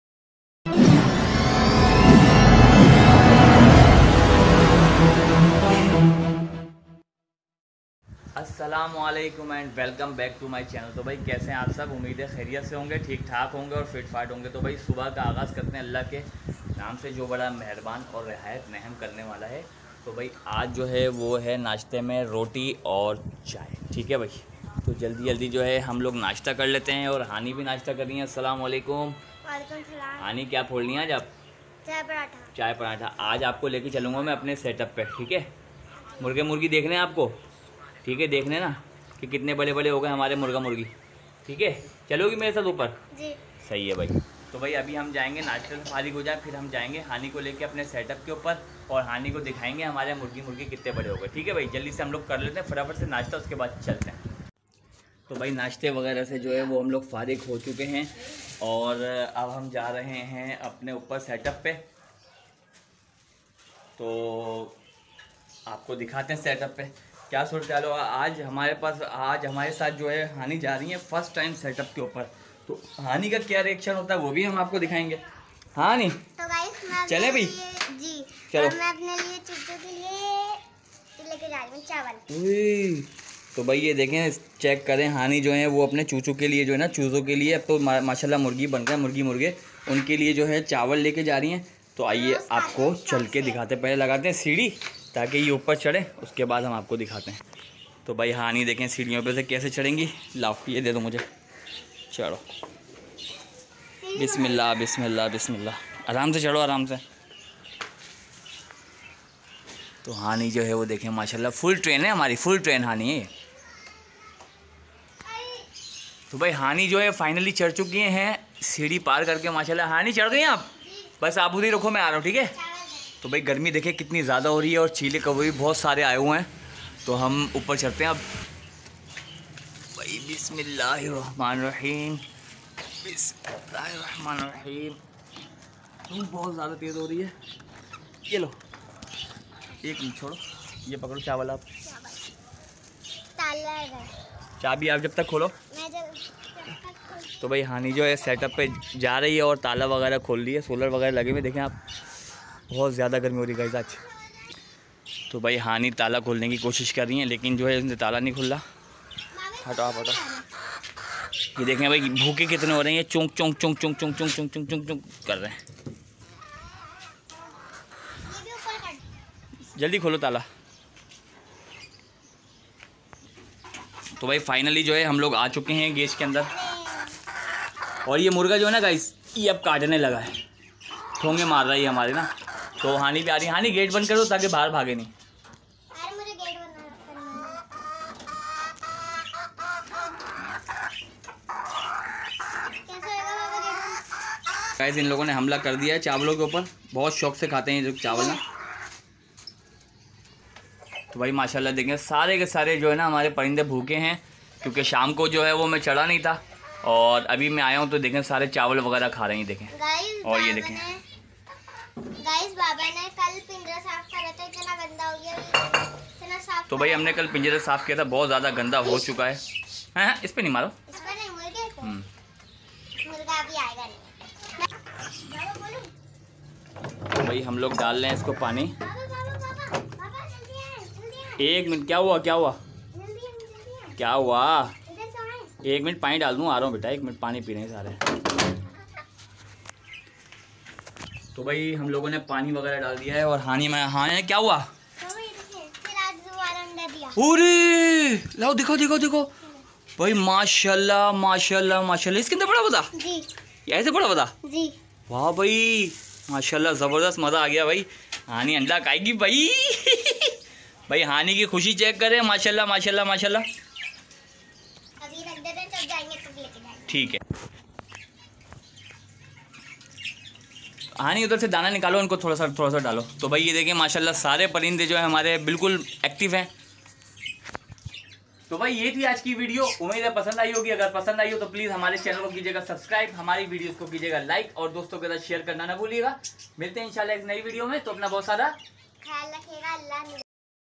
Hani ko cage ka visit krwaya || Hani happy hogai 😂😂
Hani ko cage ka visit krwaya 😁😁

Daily vlog hy simple ...hope your gues like it..